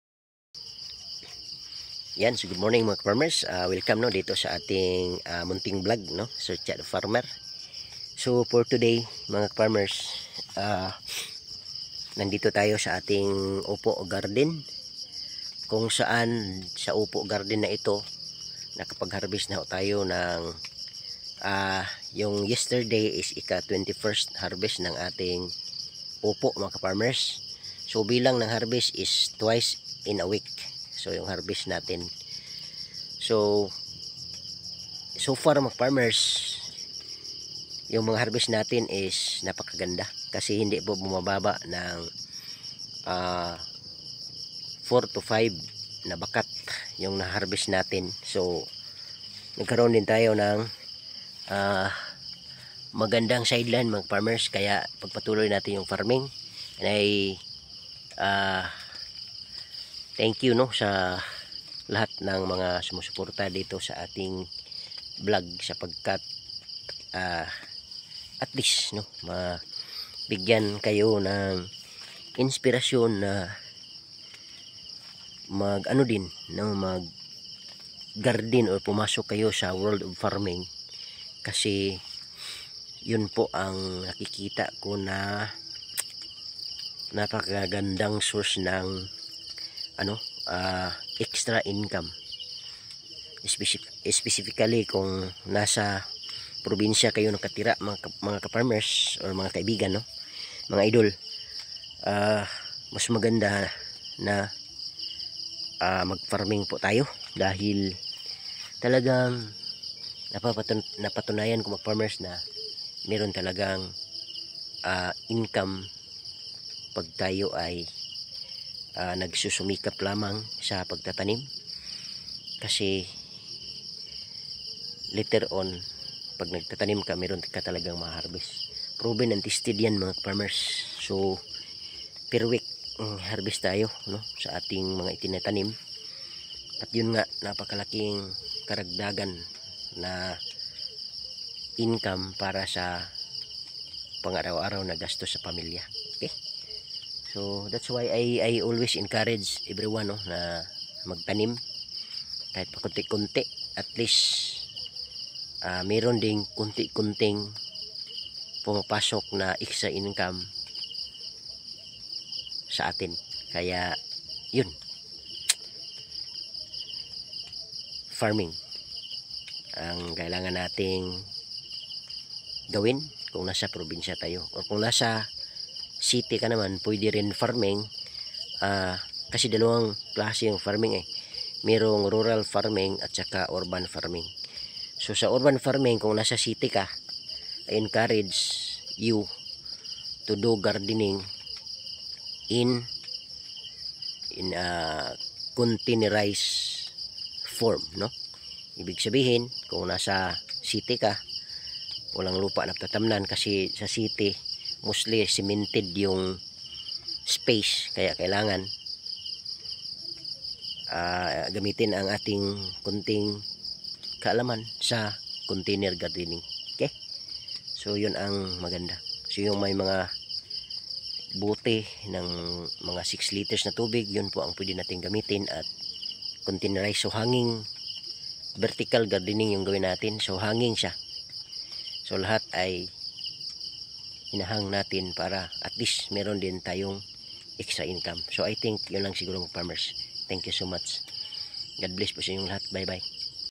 Upo Farming napakagandang source for extra income.
Upo farming as an additional source of extra income.